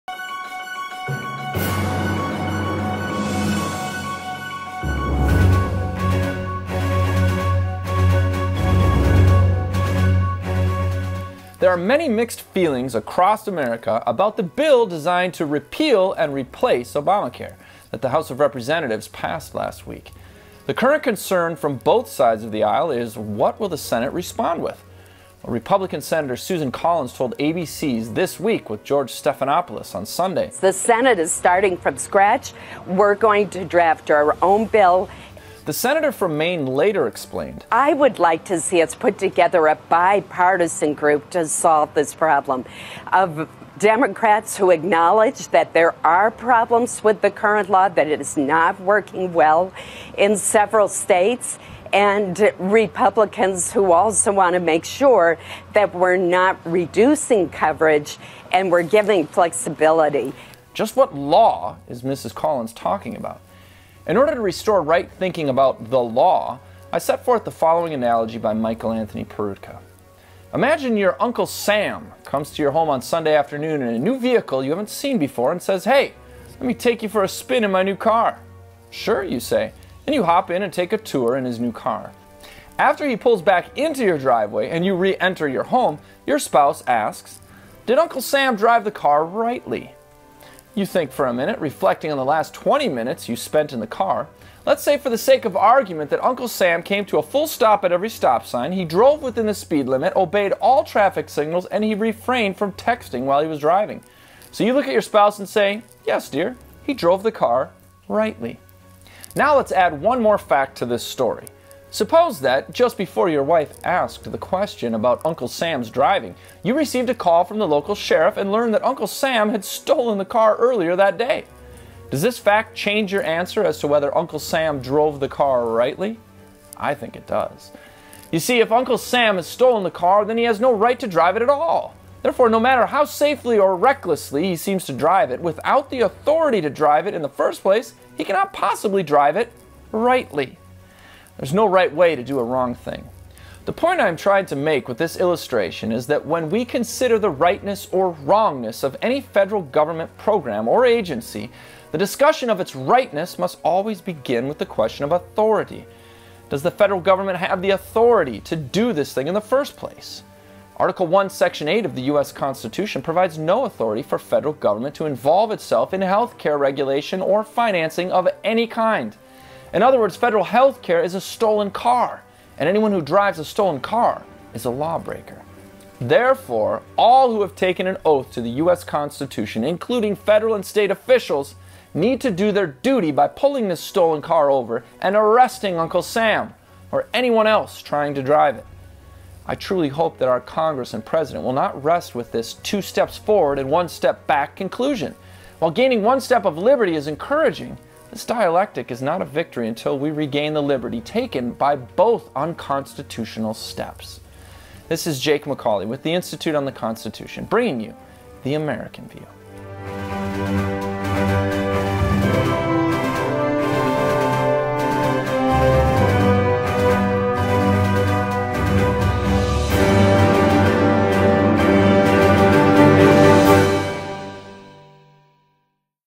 The America Healthcare Dialectic Cannot Stop Here: Jake MacAulay
There are many mixed feelings across America about the bill designed to “repeal and replace” Obamacare that the House of Representatives passed last week.

The current concern from both sides of the aisle is what will the Senate respond with?

Republican Sen. Susan Collins told ABC’s “This Week” with George Stephanopoulos on Sunday:

"The Senate is starting from scratch. We're going to draft our own bill.”

The Senator from Maine later explained, “I would like to see us put together a bipartisan group to solve this problem, of Democrats who acknowledge that there are problems with the current law, that it is not working well in several states, and Republicans who also want to make sure that we're not reducing coverage and we're giving flexibility.”

Just what “law” is Mrs. Collins talking about?

In order to restore right thinking about “the law,” I set forth the following analogy by Michael Anthony Peroutka:

Imagine your “Uncle Sam” comes to your home one Sunday afternoon in a new vehicle you haven’t seen before and says, “Hey! Let me take you for a spin in my new car.”

“Sure,” you say, and you hop in and take a tour in his new car.

After he pulls back into your driveway and you re-enter your home, your spouse asks, “Did Uncle Sam drive the car ‘rightly’?”

You think a minute, reflecting on the last 20 minutes you spent in the car.  Let’s say, for the sake of argument, that Uncle Sam came to a full stop at every stop sign, he drove within the speed limit, obeyed all the traffic signals, and he refrained from texting while he was driving.  So you look at your spouse and say, “Yes, dear, he drove the car rightly.”

Now, let’s add one more fact to this story.

Suppose that, just before your wife asked the question about Uncle Sam’s driving, you received a call from the local sheriff and learned that Uncle Sam had stolen the car earlier that day.

Does this fact change your answer as to whether Uncle Sam drove the car rightly?

I think it does.

You see, if Uncle Sam has stolen the car then he has no right to drive it at all.  Therefore, no matter how safely or recklessly he seems to drive it, without the authority to drive it in the first place, he cannot possibly drive it “rightly.”

There is no right way to do a wrong thing.

The point I am trying to make with this illustration is that when we consider the rightness or wrongness of any federal government program or agency the discussion of its “rightness” must always begin with the question of authority.  Does the federal government have the authority to do this thing in the first place?

Article One, Section Eight, of the U.S. Constitution provides no authority for the federal government to involve itself in health care regulation or financing of any kind.  In other words, “Federal Healthcare” is a stolen car.  And anyone who drives a stolen car is a lawbreaker.

Therefore, all who have taken an oath to the U.S. Constitution, including federal and state officials, need to do their duty by pulling this stolen car over and arresting “Uncle Sam” or anyone else trying to drive it.

I truly hope that our Congress and President will not rest with this “two steps forward, one step back” conclusion. While gaining one step of liberty is encouraging, this dialectic is not a victory until we regain the liberty taken by both unconstitutional steps.